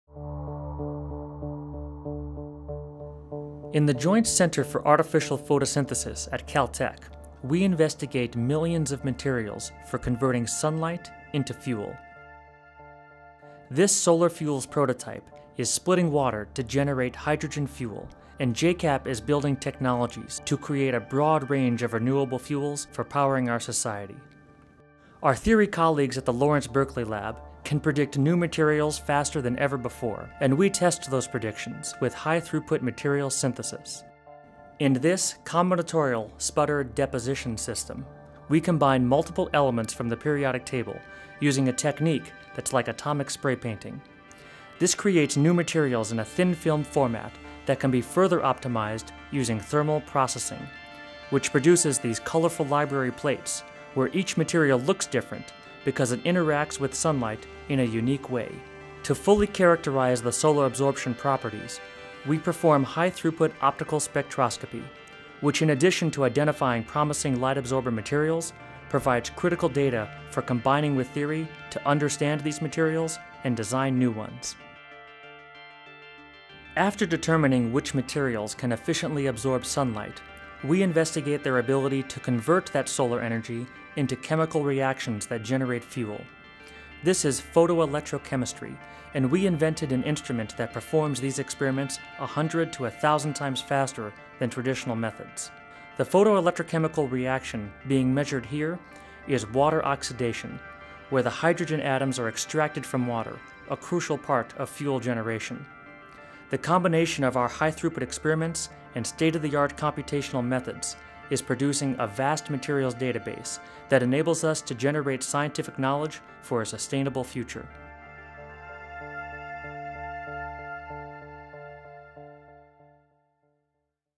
Caltech and JCAP Discover Solar Fuel Materials
A look inside the labs at the Joint Center for Artificial Photosynthesis reveals the inventions and processes that yield groundbreaking discoveries in solar fuel production.